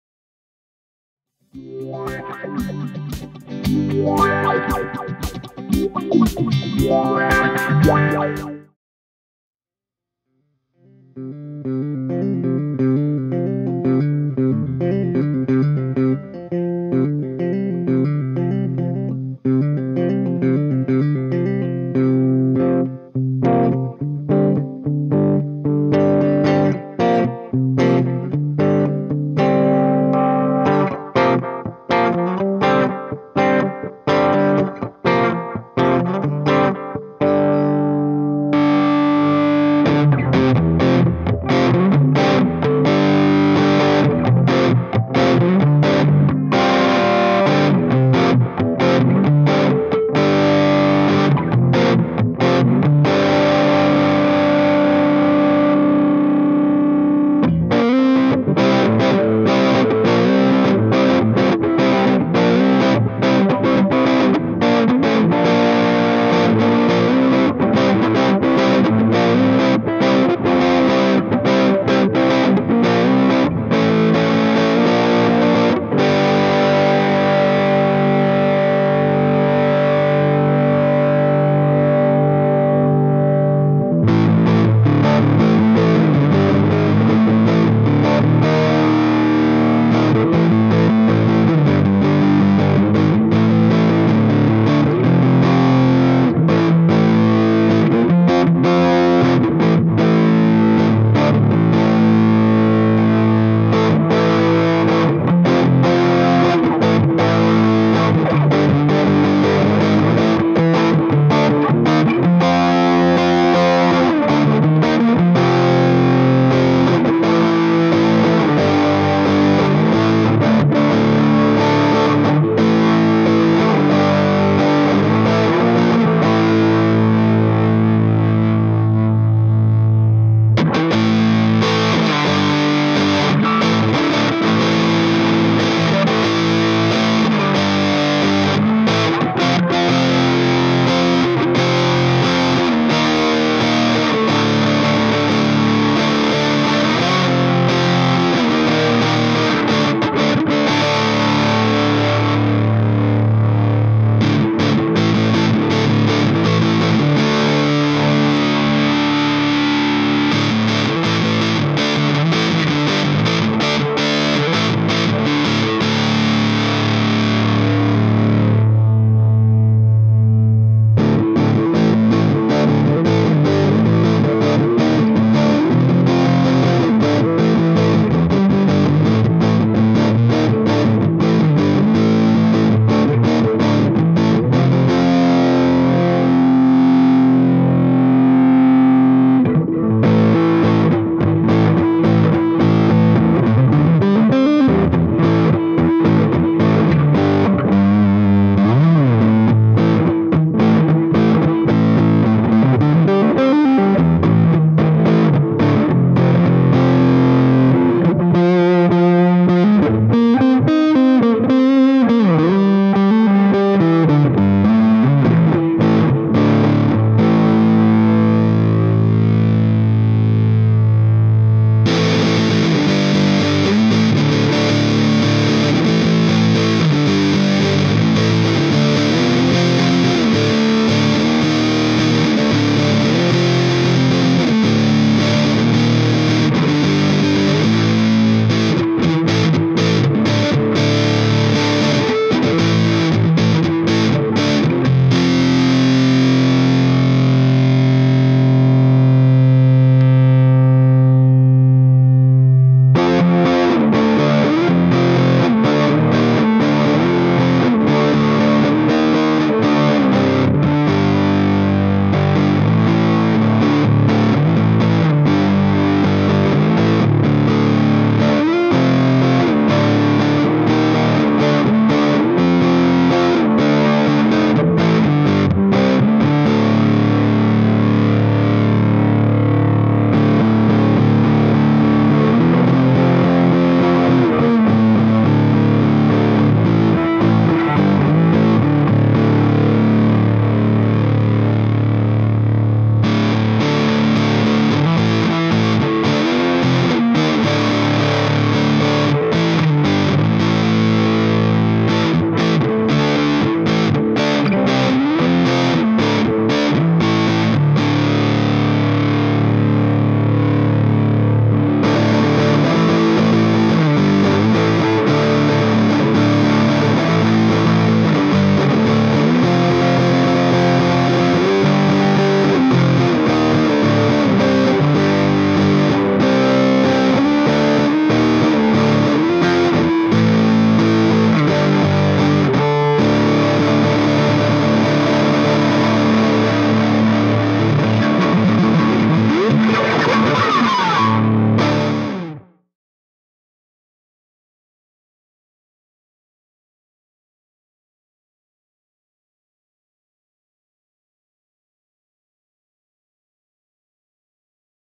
Mistweaver Effects The Serpent Fuzz
This is a demo video of The Serpent Fuzz effects pedal from Mistweaver Effects performed and produced by Pedal of the Day. for more effects pedal reviews and demos, and be sure to check out more of the videos on our YouTube channel as well. Cheers!

Recorded (in stereo) with:

PHRED Instruments Ernesto VH3 Koa Brown Burst loaded with Seymour Duncan ’59 SH-1n and SH-1b Humbucker pickups
Benson Amps Monarch + Mesa/Boogie Mark 5:25